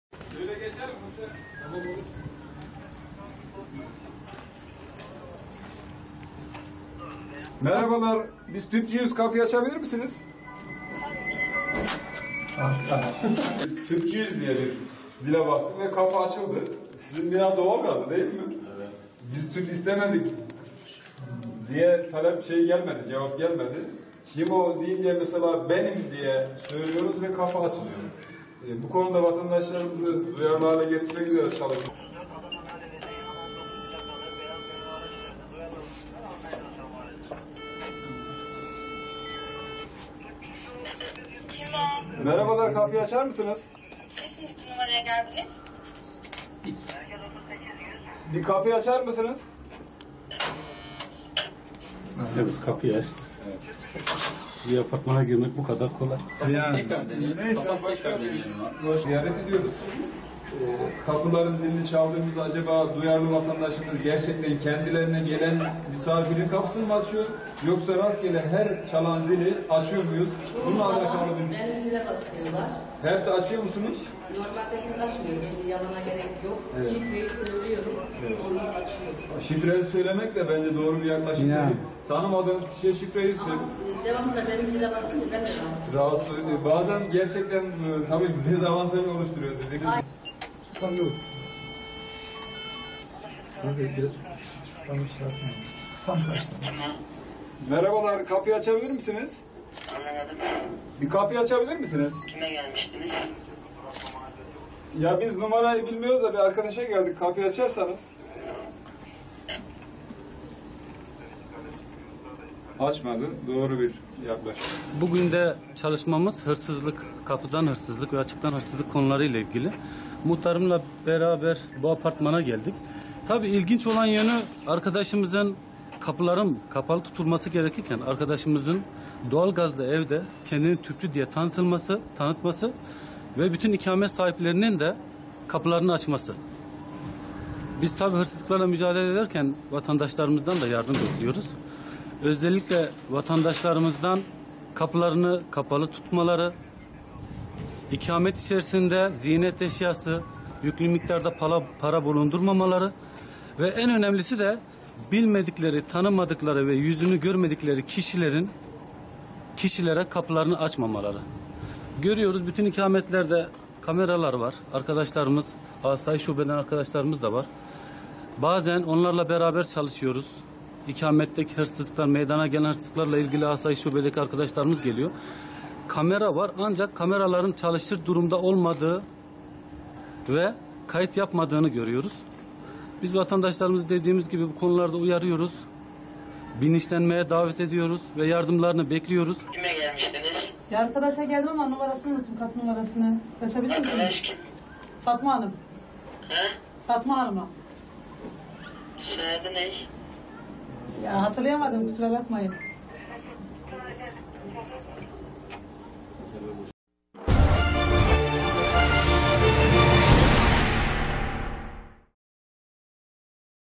Kahramanmaraş'ta 'kim o' sorusuna polis 'tüpçü' dedi, kapı açıldı!
Kahramanmaraş'ta polis, hırsızlara karşı dikkatli olunması için ilginç bir çalışmaya başlattı. Apartman zillerini çalıp tüpçü olduğunu ifade eden polisler, kapıyı açan vatandaşları uyardı.